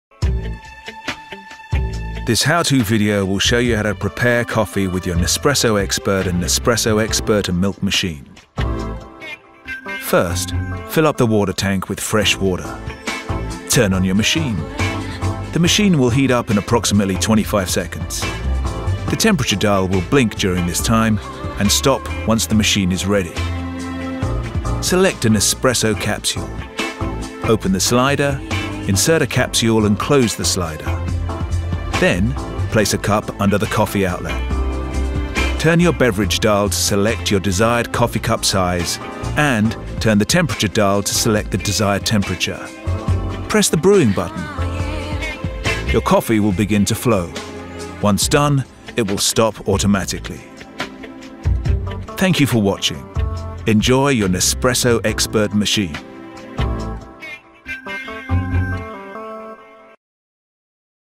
New Nespresso Expert - How to Video - Preparing Coffee in 3 easy steps
Nespresso shows you in three easy steps how to prepare a coffee with your new machine.  Discover the New Nespresso Expert and Nespresso Expert&milk machine and its unique features